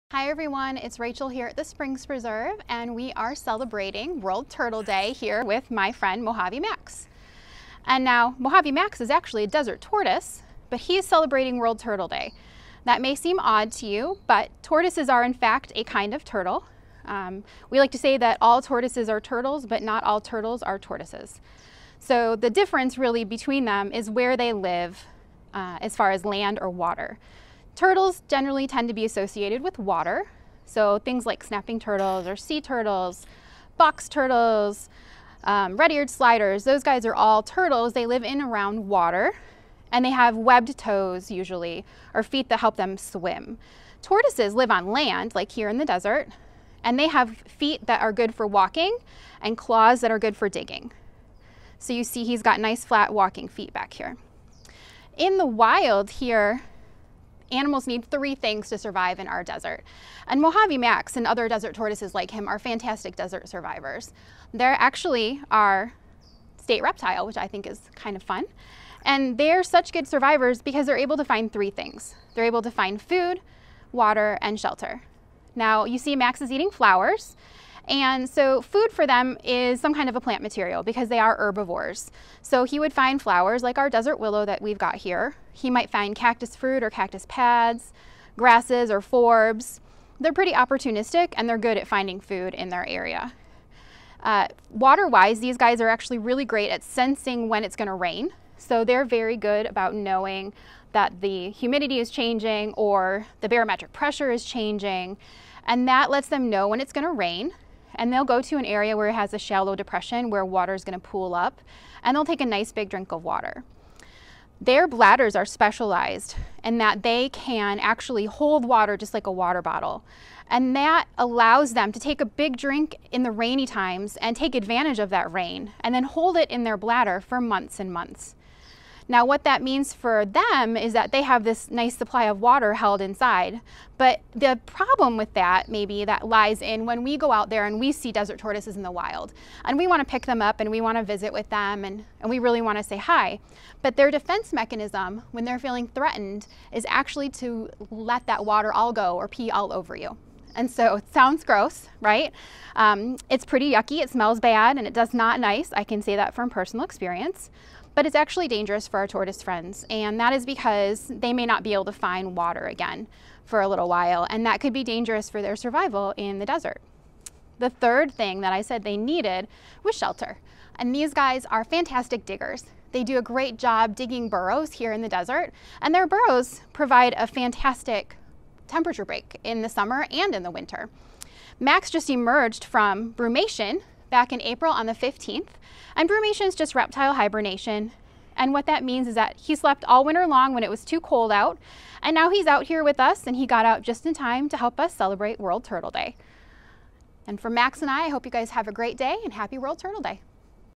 Munch and Learn with Mojave Max | World Turtle Day
Happy World Turtle Day! 🐢 “Shellebrate” by visiting Mojave Max and his friends at the Springs Preserve's Desert Tortoise Habitat.

Learn more about Mojave Max at MojaveMax.com.

Learn how you can help protect turtles and tortoises and their disappearing habitats at WorldTurtleDay.org.